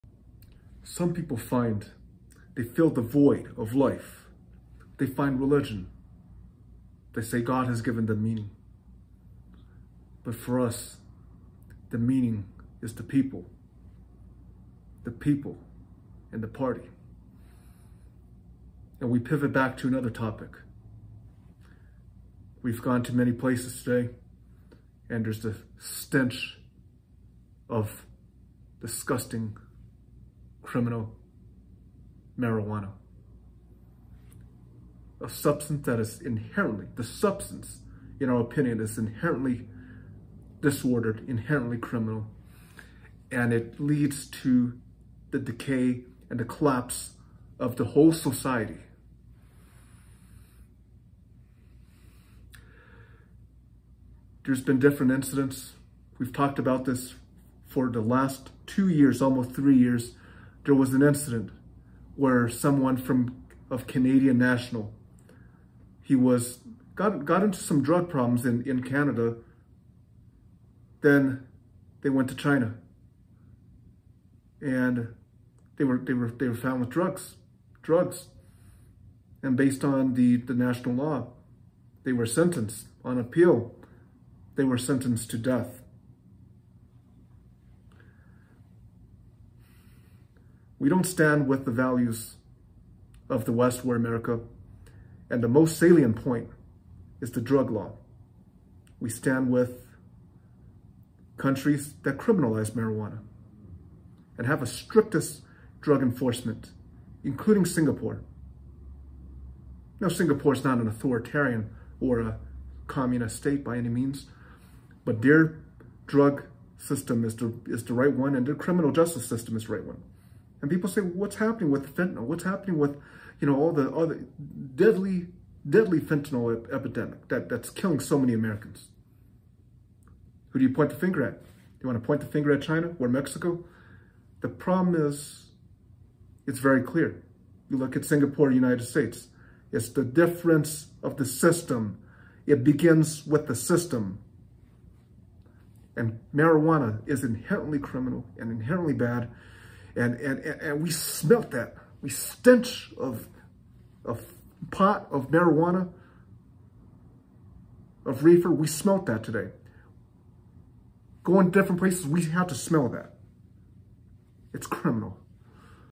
Forced To Smell Criminal Marijuana. Evil Permissive Liberal Drug Use Culture Invading All America.
The old regime was overthrown with just cause. I want to see every dance hall and night club closed by law: especially rave clubs & night clubs playing western music. The people’s armed police of the People’s Republic of China must systematically close every rave night club from provincial city to provincial city & distribute pamphlets on socialism and socialist values. The vile evil KMT allowed dance halls and night clubs to flourish with degenerate low cut revealing dress styles of the so called “Republican Era”. Our morally superior COMMUNIST revolution put an end to this vile disorder. The upright moral superiority of the CPC is self-evident. There is an nude beach representing a very vile sort of culture at odds with socialist core values and the creation of the most pure and spiritually transcendental militarized social order of society shunning the degenerate 1920s dance ball rooms, capitalism and commercialism, and drug use. The stench of reefer, pot, Mary Jane, or marijuana is unmistakable. This stench pervades the streets and beaches from Nevada to California: marijuana invasion of hearts and minds and will end up destroying the entire United States by criminal drug use. Tobacco is also a grave social evil, alongside the night clubs and dance halls pervading formerly socialist societies. In the 1950s and up to the 1970s there were no night clubs but dedicated study sessions inculcating the correct values of political virtue by the Party. The vile Kuomintang or KMT 国民党 allowed these degenerate ball rooms to flourish and the elites attended the evil foreign dances with the people corrupted by swing music. The revolution must close down all the vile dance halls: starting with the most core rotten city of Shanghai with its connections to foreign culture and hostile influences. All dance halls, ball rooms, must be closed.

As a matter of fact a Canadian national was duly sentenced to death China for drug smuggling with global outctry in accordance to Article 147 of the law on drugs. Long Live The Communist Party of China and our seperate CCP socialist core values.